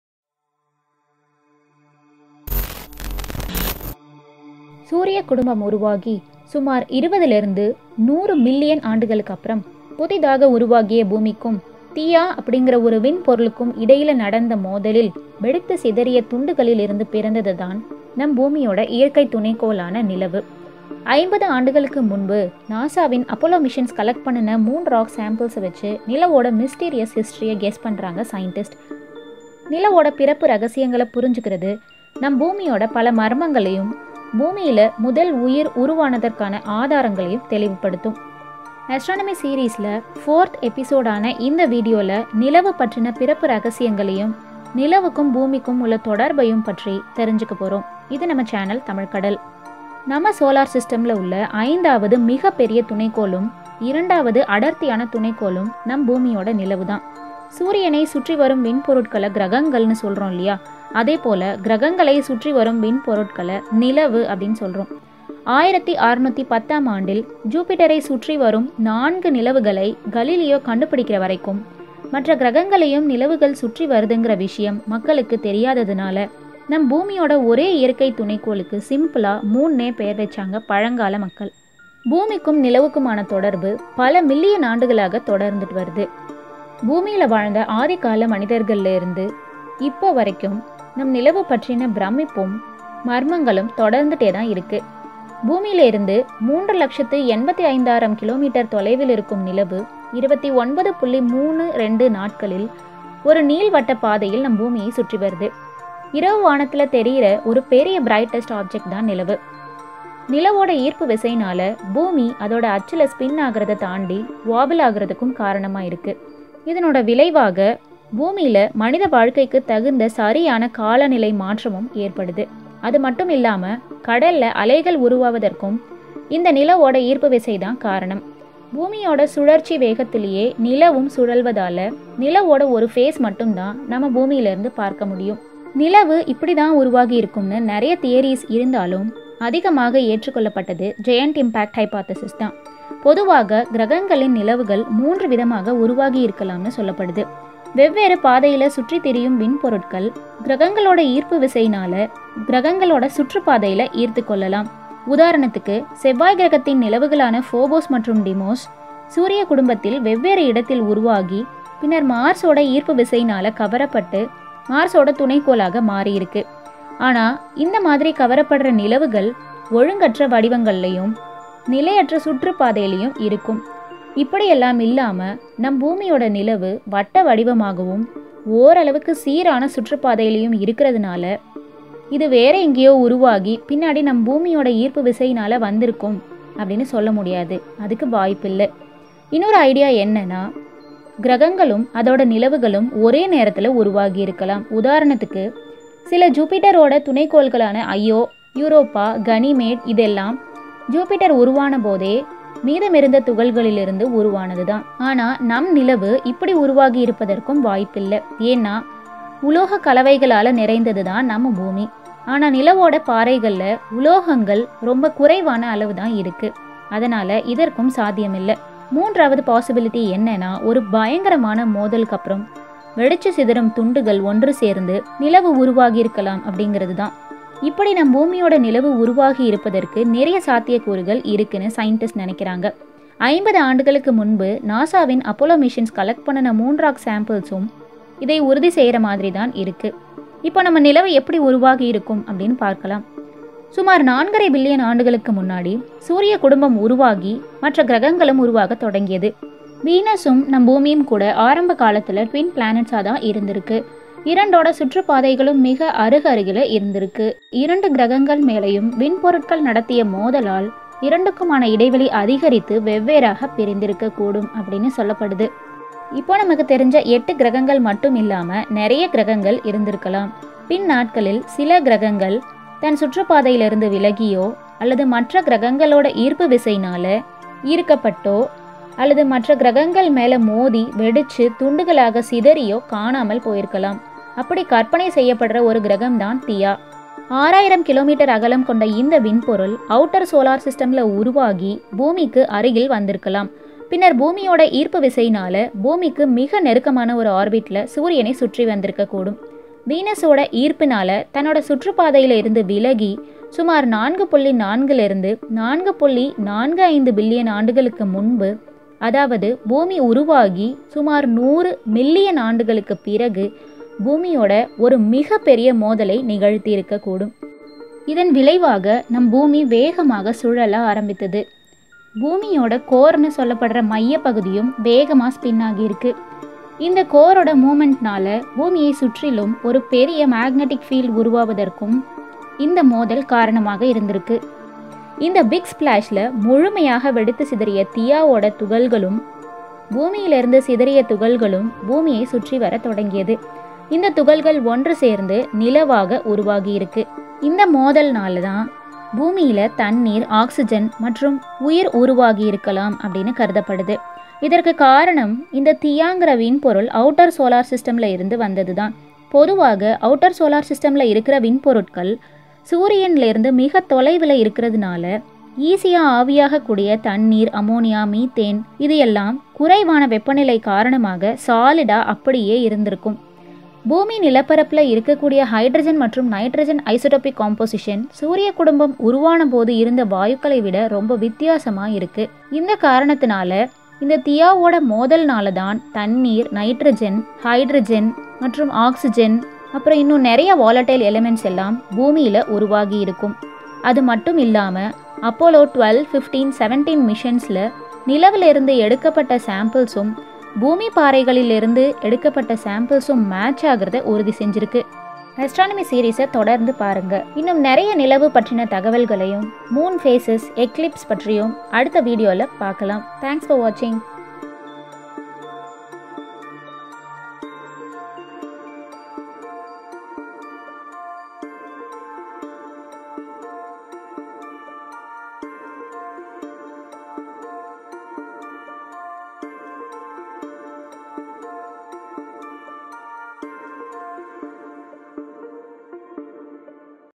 Mysterious Birth and Formation of Moon| Astronomy series Episode 4|Water| Oxygen|Life in earth|Tamil
Mysterious Birth and Formation of Moon, Collision of Theia with Earth,Origin of Life,Water, Oxygen(Tamil)-Astronomy series Episode 4
(@Behind Earth)

This video explains the birth and formation of moon some billion years ago when there was a collision of Theia with Earth. And only after this collision of theia with earth, water oxygen and life in earth begins. This video is a visual treat for spacescience lovers.

4.5 billion years ago after the formation of sun and 20 planets from the solar nebula many debribs left in the solar system which makes collision with one another and with newly formed planets. Such a collision takes place between the proto planet theia and earth,100 million years after the formation of earth.

This collison gave birth to moon from the debris ejected from both earth and theia after collision. Due to this collision earth started to spin faster with a wobbling motion. And this collision brought oxygen water and life in earth.

Keep supporting our channel "Tamil Kadal"